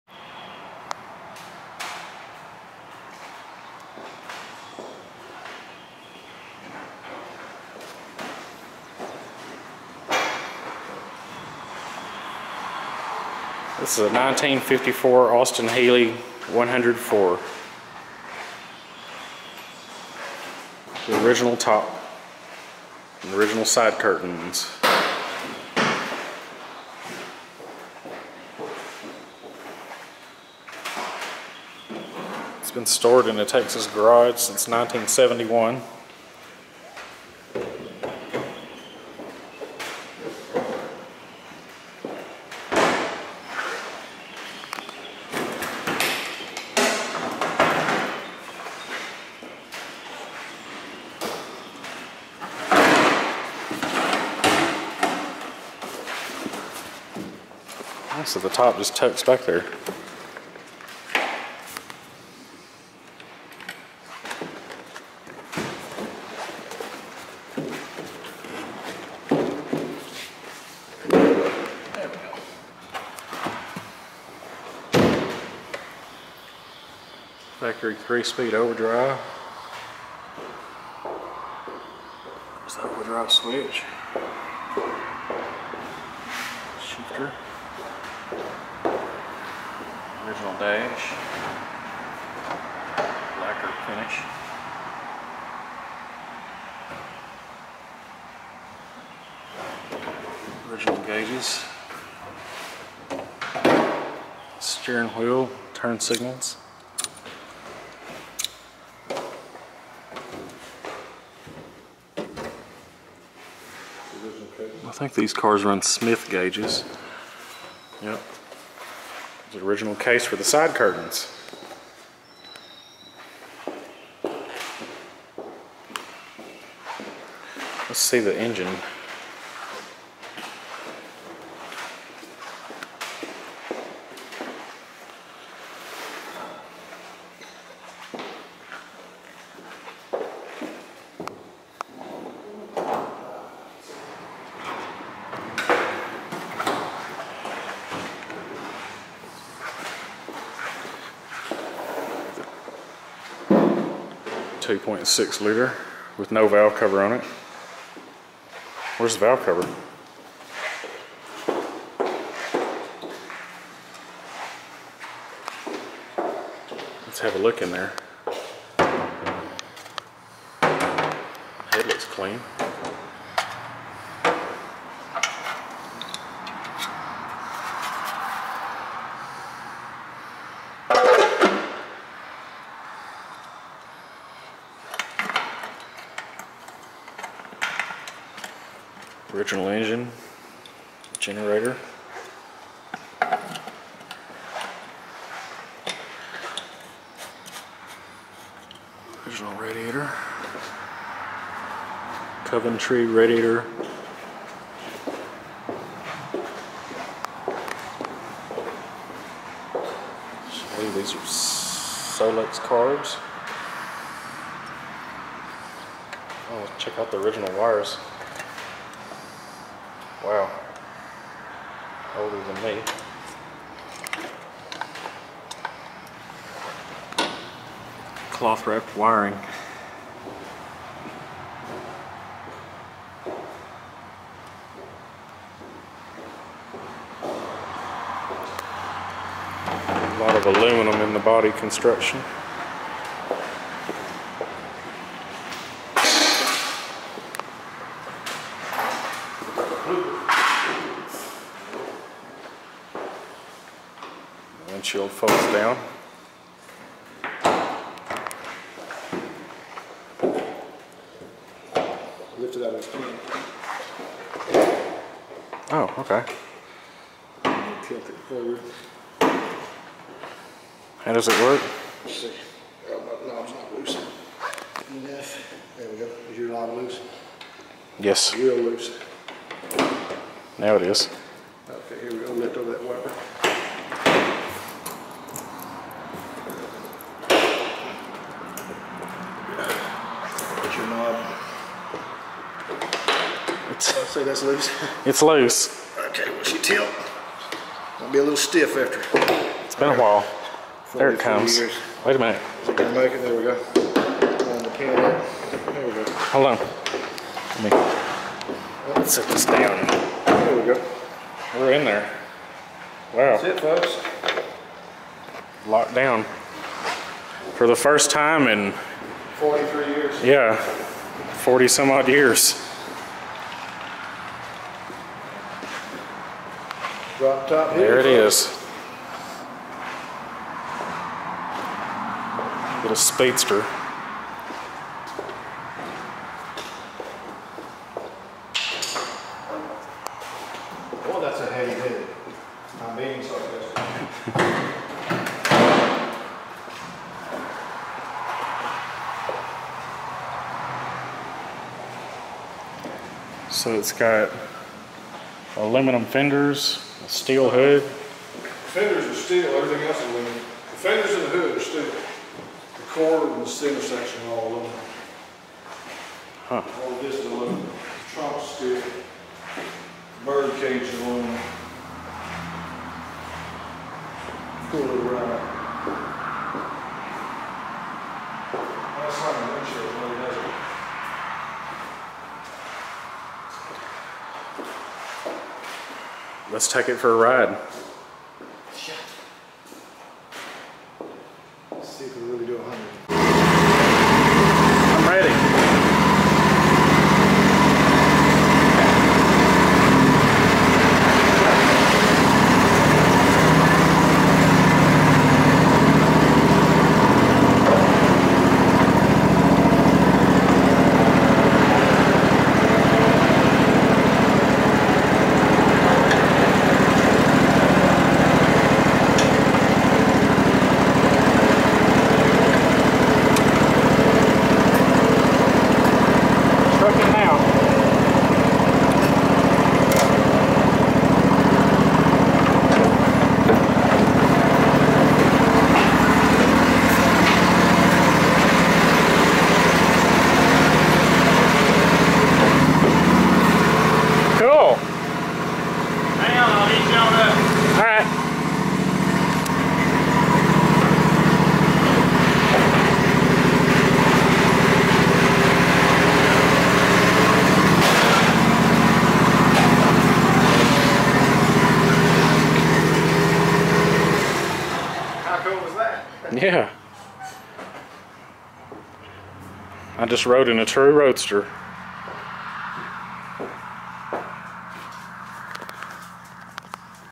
1954 Austin-Healey 100-4 Texas Garage Find
43+ years sitting in a Texas garage, a 1954 Austin-Healey 100-4 finds a new lease on life with Street Machines. Enjoy the video tour of this special classic car - Sam